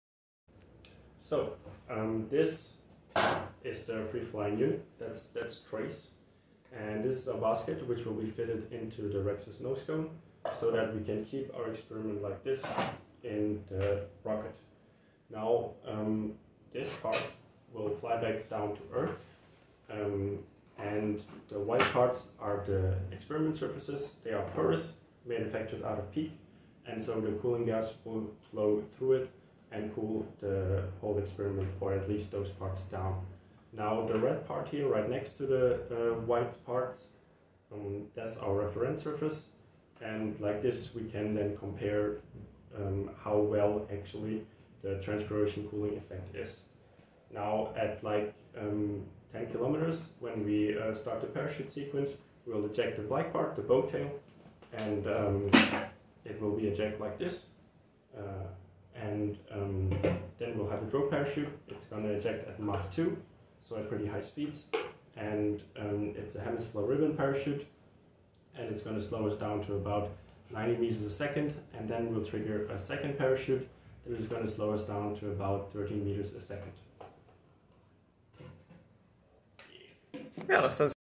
Transpiration Cooling Experiment, TRACE - Vehicle overview
TRACE is a Rexus mission of Space Team Aachen, a student team from the University of Aachen. Their capsule will ride the REXUS rocket up and perform an active cooling experiment on the down leg. In this video, the project lead, Nicolas Heyn, provides an overview of the vehicle.